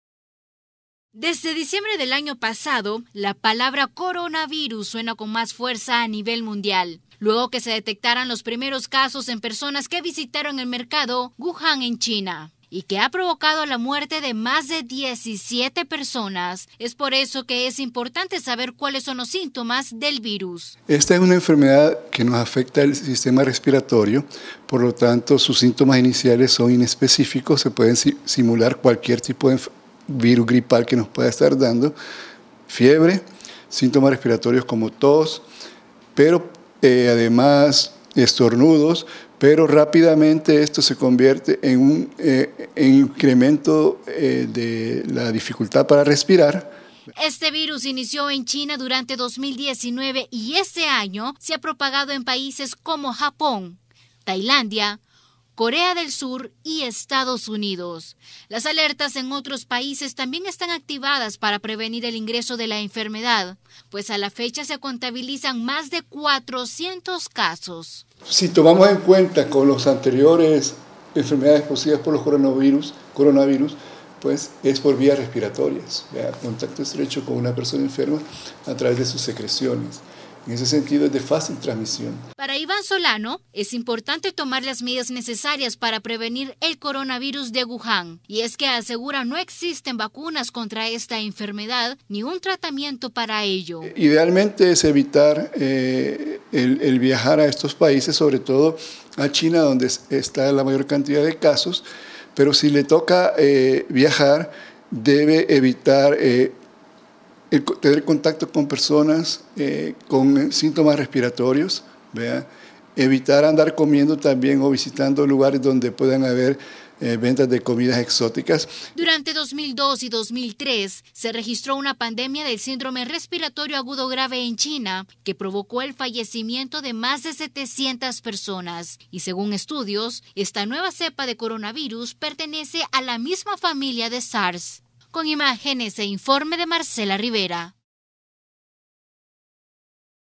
¿Qué es el coronavirus y por qué preocupa su expansión?
El virus provoca neumonía y puede transmitirse de persona a persona.
¡Visítenos!